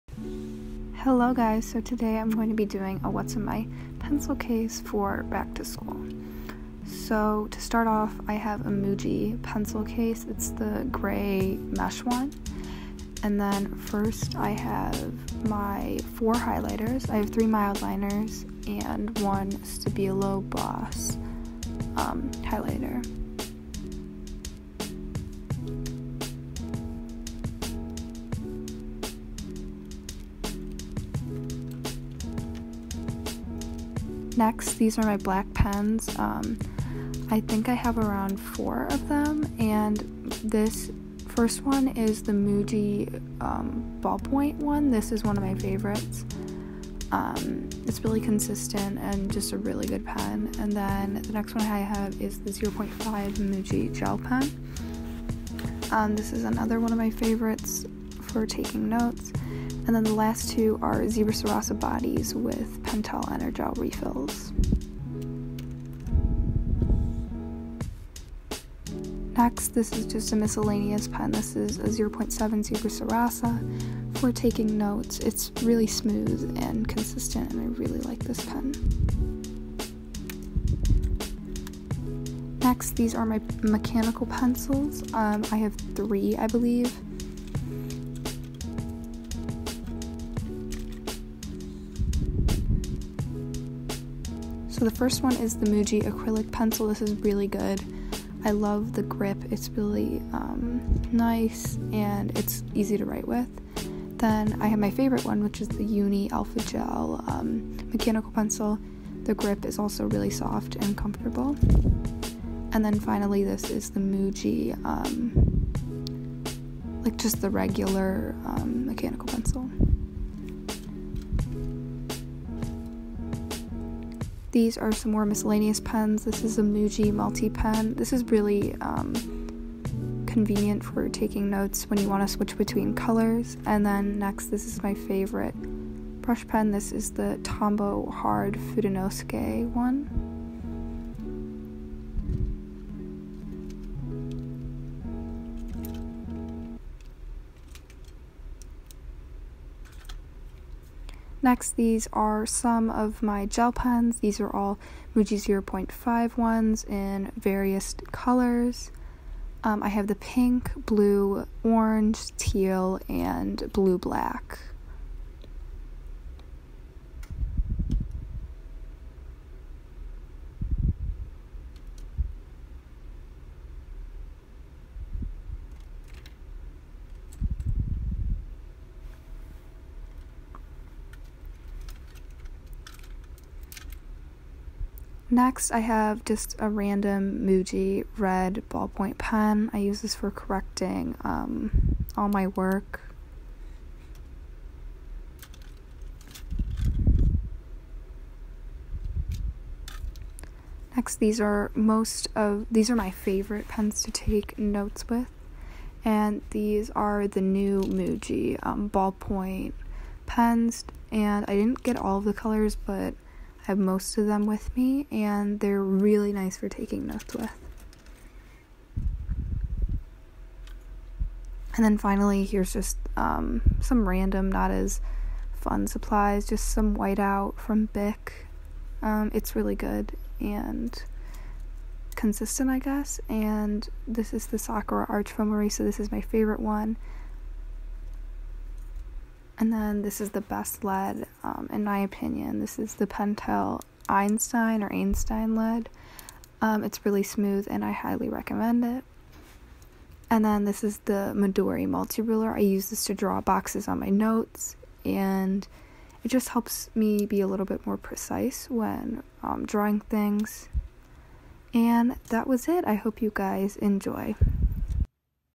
what’s in my pencil case 2019 | vividstudies
hi guys welcome back to another video!! thank you for all the love on the last one and i’m so happy to be making more videos again!!!

all stationary purchased in this video is from jetpens.com or mujiusa.com💕

Musician: Jef